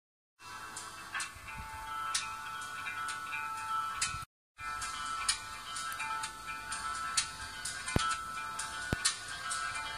Handmade beetle wing ear cuff
This is my Handmade ear cuff that I created for my makeup made of Beetlewings. Applied glitterd and crystals do get better effect.
XOXO